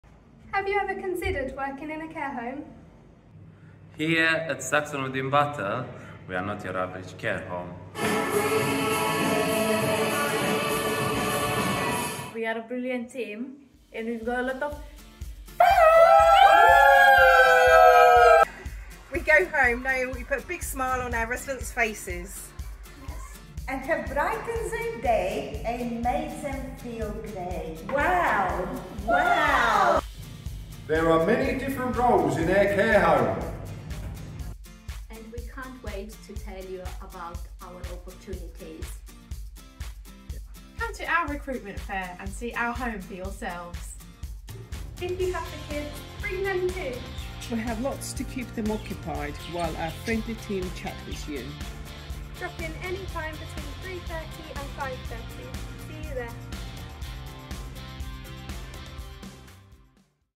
Pop along to Saxonwood care home's recruitment fair! Thursday 13 July 2023, 3.30pm to 5.30pm
Pop along on Thursday 13 July 2023 anytime between 3.30pm and 5.30pm to find out about the roles we have available, what it's like to work at Saxonwood and to meet our friendly team. No booking needed and children are welcome too, we have lots to keep them occupied!